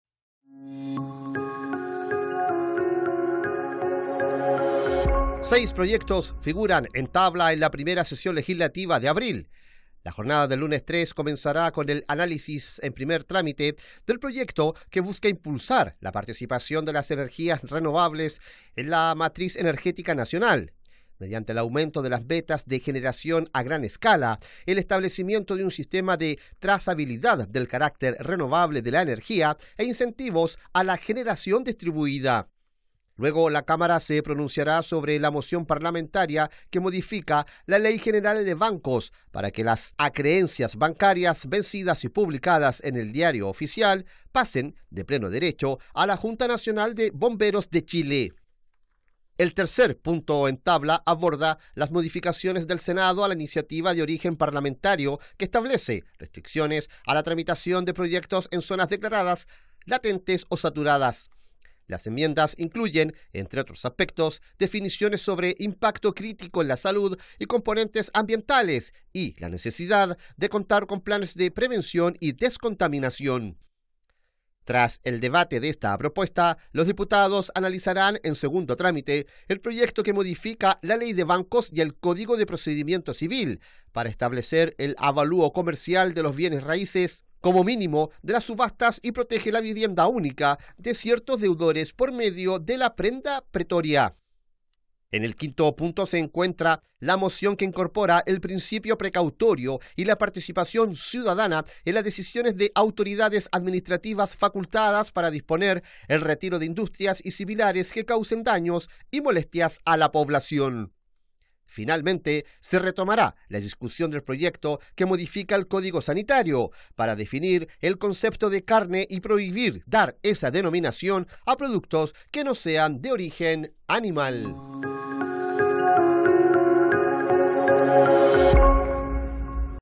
Seis proyectos en tabla en primera sesión legislativa de abril
La jornada en Sala la encabeza la iniciativa del gobierno que pretende impulsar la participación de energías renovables en la matriz energética del país.